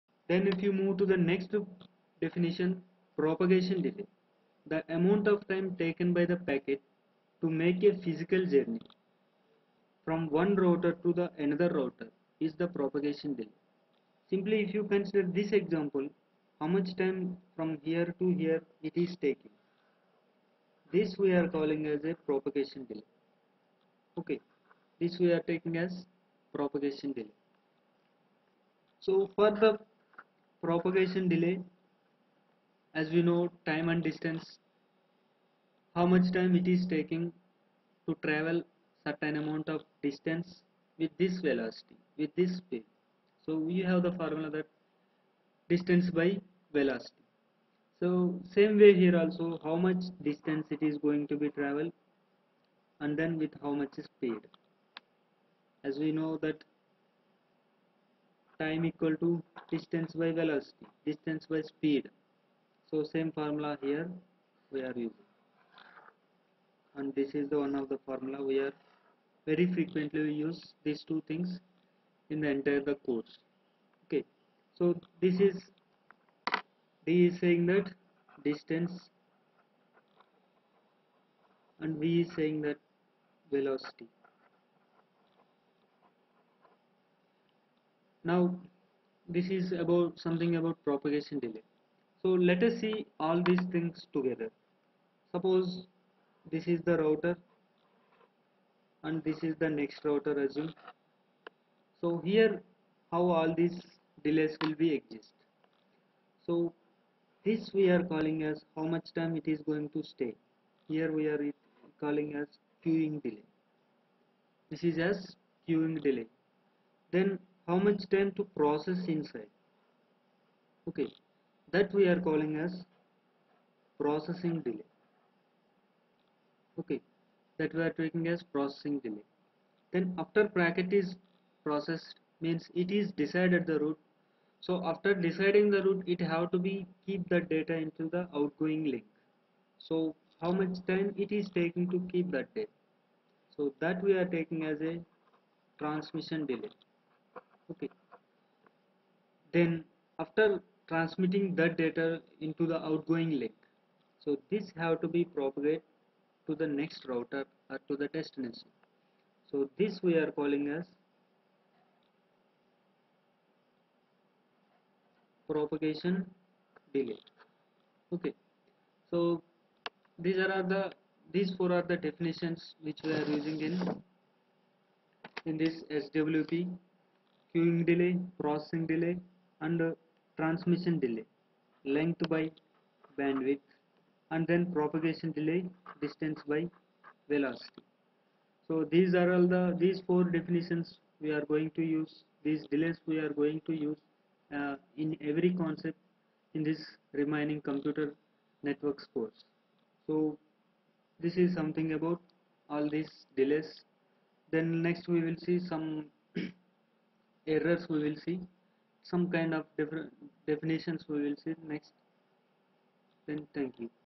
Computer Networks: Propagation Delay
Propagation Delay explained. To ask your doubts on this topic and much more,